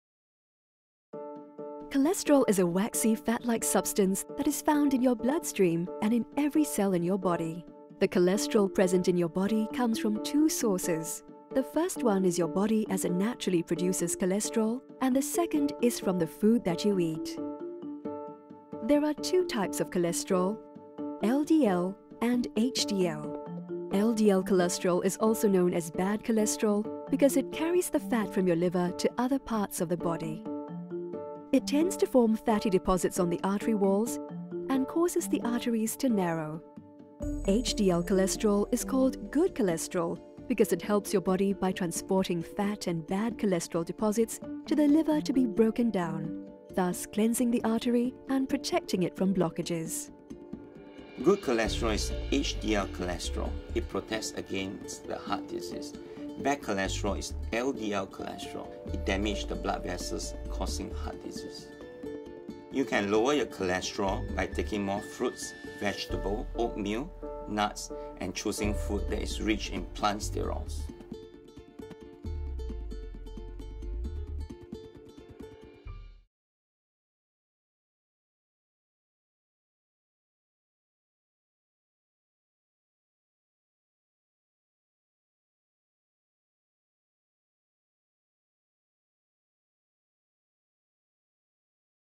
Heart Truths Episode 2: What is Cholesterol? (English)
What is cholesterol? Understanding cholesterol with Heart Truths Episode 2

In this video, we explain what cholesterol is and the difference between good and bad cholesterol.

Copyrights owned by Malaysia Dairy Industries Pte Ltd.